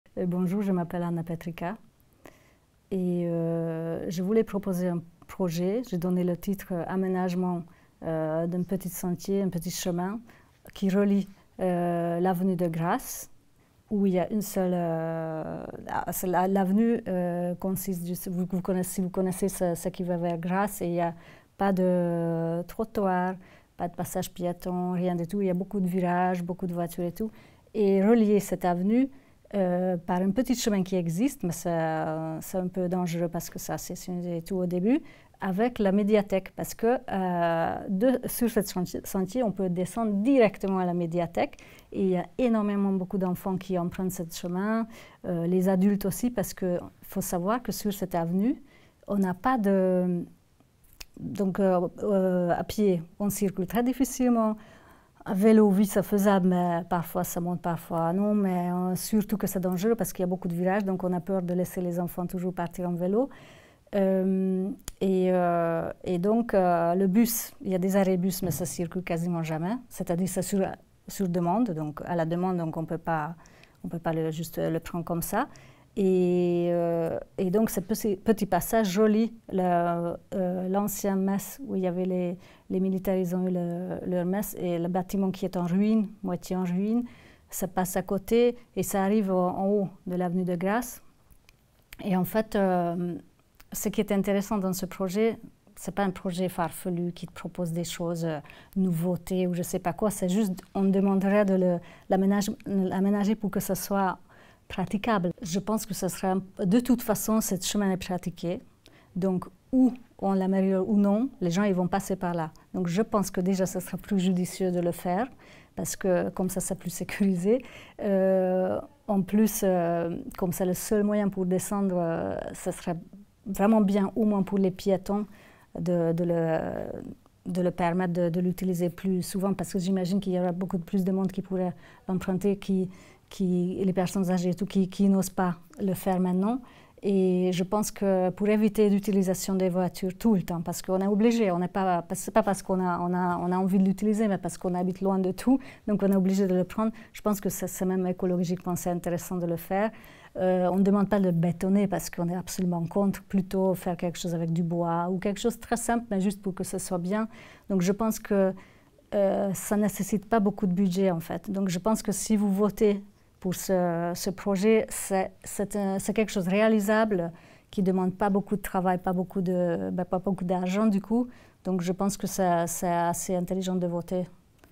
Chemin reliant avenue de Grasse à la médiathèque: Anna PETRIKA, Je vote pour mes idées préférées !
BUDGET PARTICIPATIF

Du 1er au 31 octobre, les dracénois âgés d'au moins 11 ans, ainsi que les personnes travaillant à Draguignan, sont invités à voter pour leur projet favori sur un site dédié : jeparticipe.ville-draguignan.fr

Ces projets, visant à améliorer la vie quotidienne à Draguignan, sont portés par 17 individus ou associations.
Un budget de 100 000 € sera alloué à la réalisation de ceux ayant obtenu une majorité de votes.

Certains porteurs de projet sont venus nous exposer le leur en détail dans 6 courtes vidéos successives : à vos écrans !